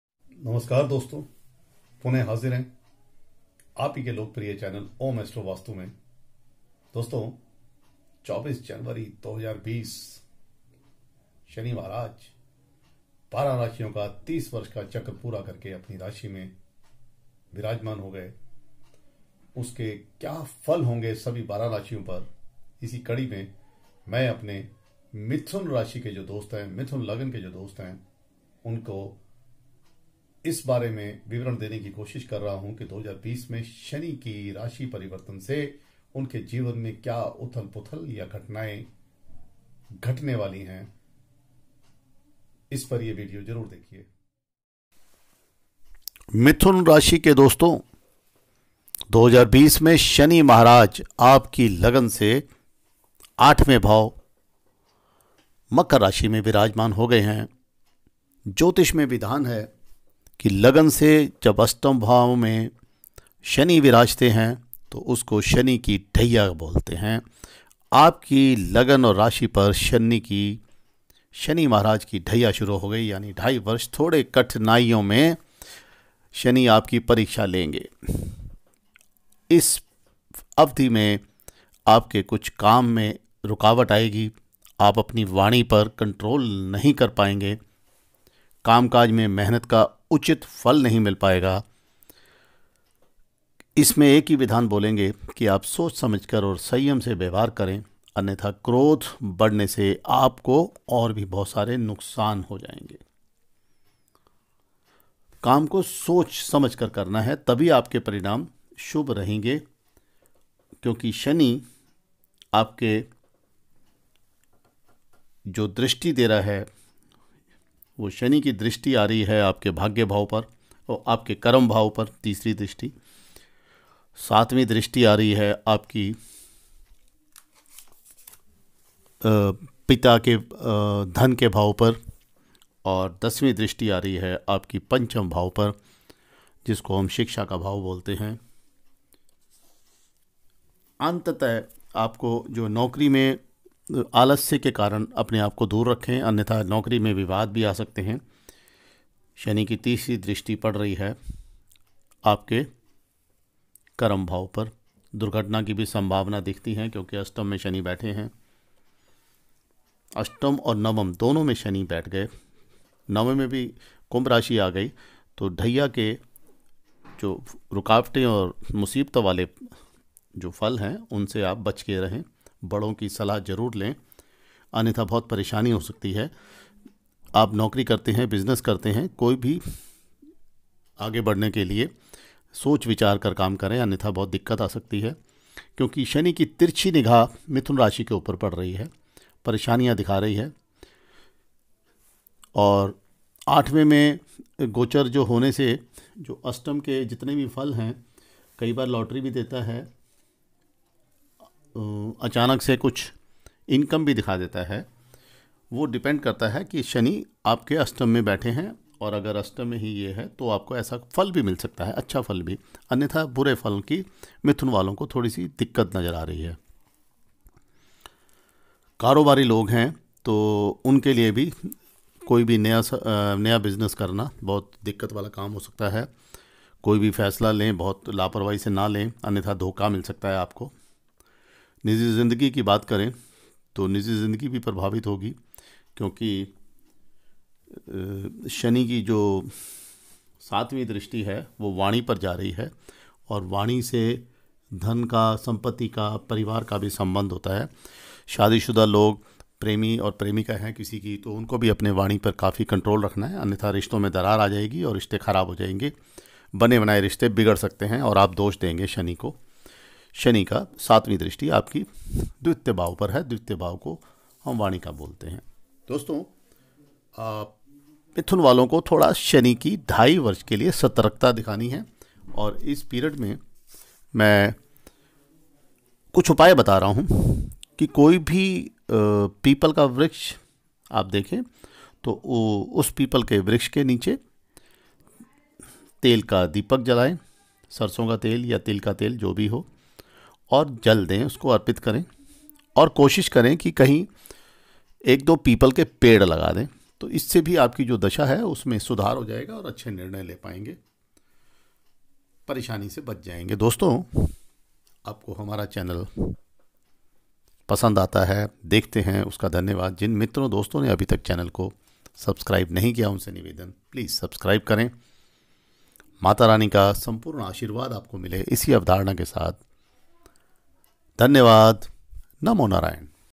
मिथुन/GEMINI को शनि गोचर 2020के फल, शनि की ढैया लग गई,शनि लेंगे ढाई साल परीक्षा
शनि 2020में मिथुन पर ढाई वर्ष की ढैया लेकर बैठ गए,ashtam ke shani arthik rup se pareshan karenge,upay kar len.